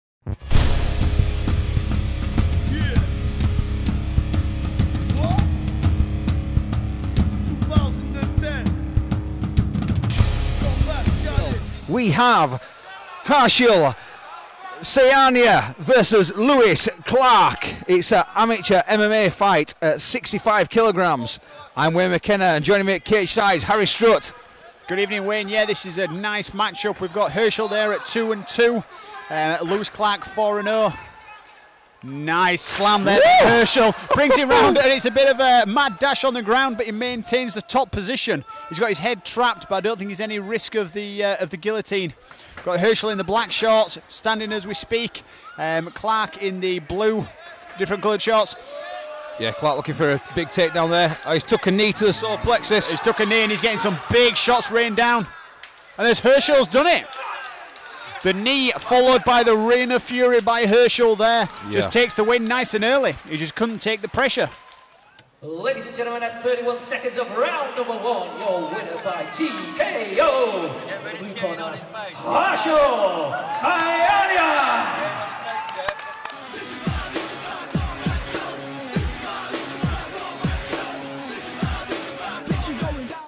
PRECISION SHOT ENDS FIGHT - HARSHIL SAYANIA V LEWIS CLARKE. CAGED STEEL
No recovery from such a solid knee to the Liver.

Please help support the YouTube Channel by sharing the clip on your wall, watching and liking it.